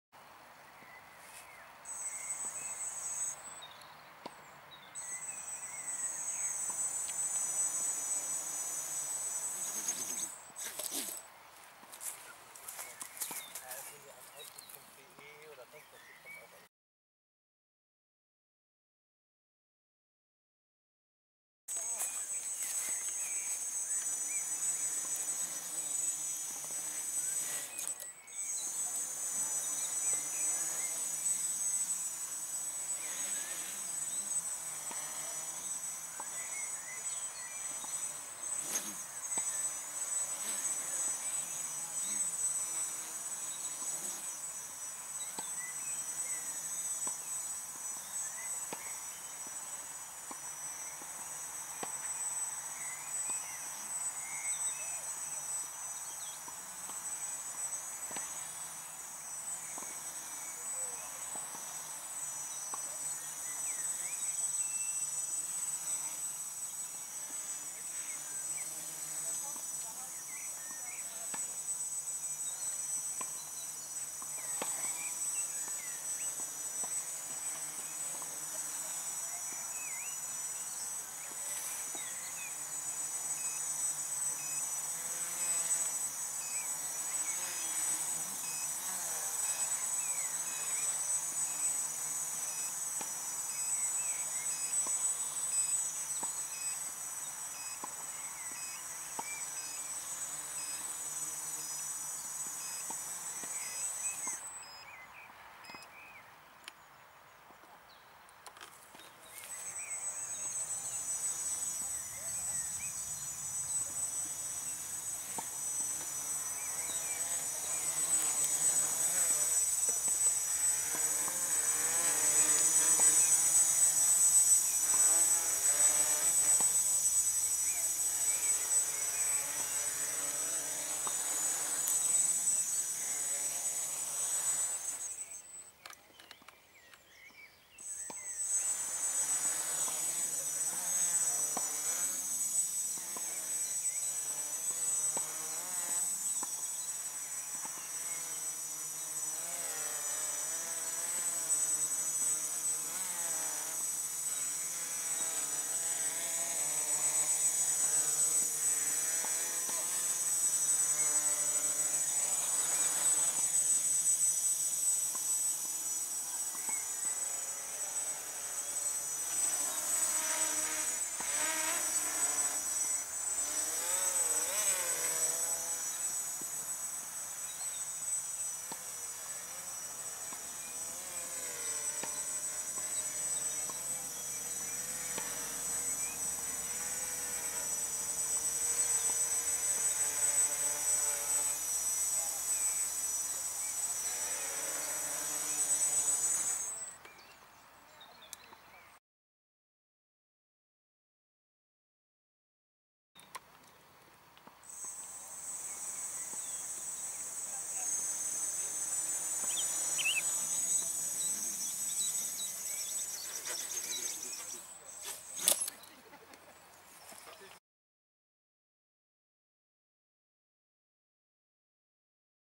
F330 QUADCOPTER - Crazy Yaw (Hobbyking i86 Multi-Rotor Control Board)
Hobbyking i86 Multi-Rotor Control Board
           - Very unstable Yaw.
         It will just keep rotating.
       Hard to control Quadcopter.